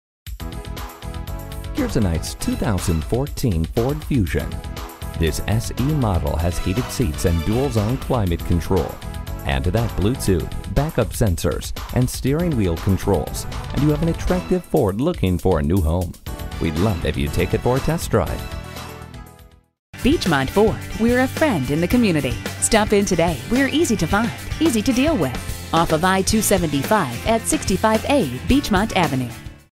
2014 Ford Fusion Cincinnati Dayton, OH C14-129 - SOLD
Year: 2014
Make: Ford
Model: Fusion
Trim: SE
Engine: 4 Cyl. 1.5 L
Transmission: Automatic
Color: Oxford White
Interior: Charcoal Black
Stock #: C14-129

Address:
600 Ohio Pike